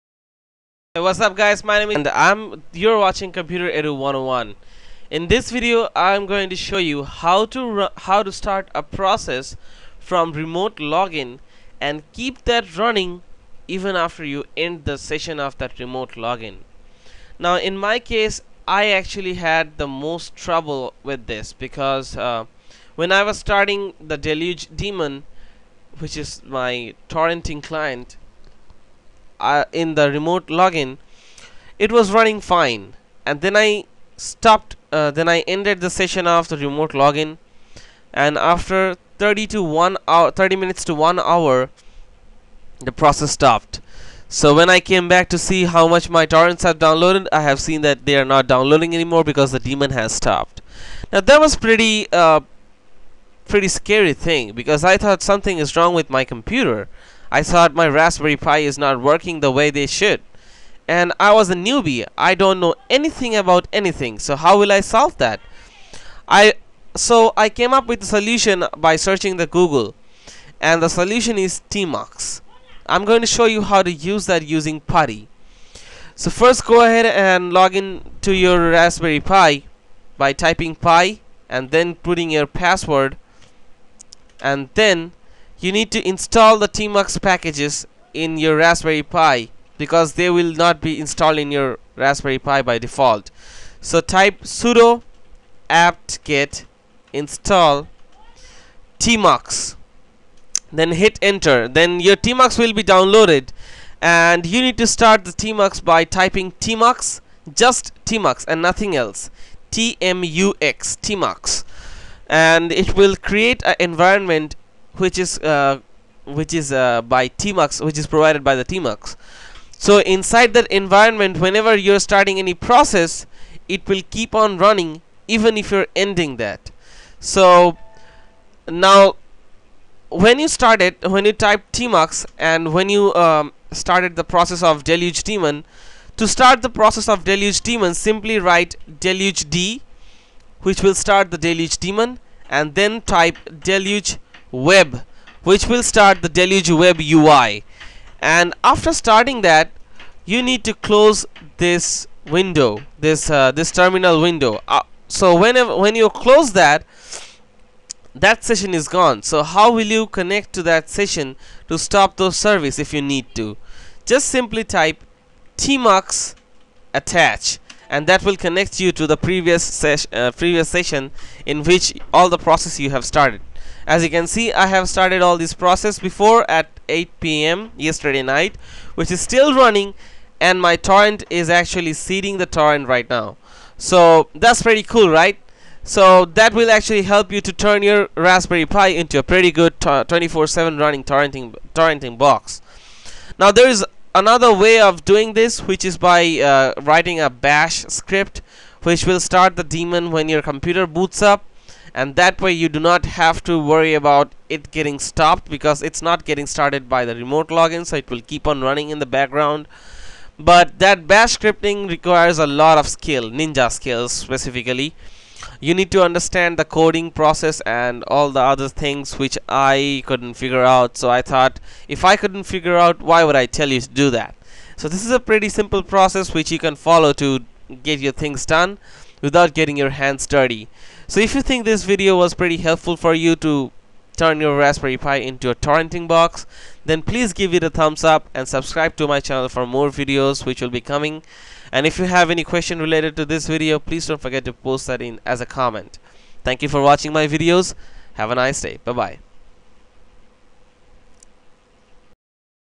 How to start a process from remote login and keep that running even wehn your remote session ends
In this video i will teach you how to start a process from remote login in raspberry pi and keep that running even when your remote login sessions ends.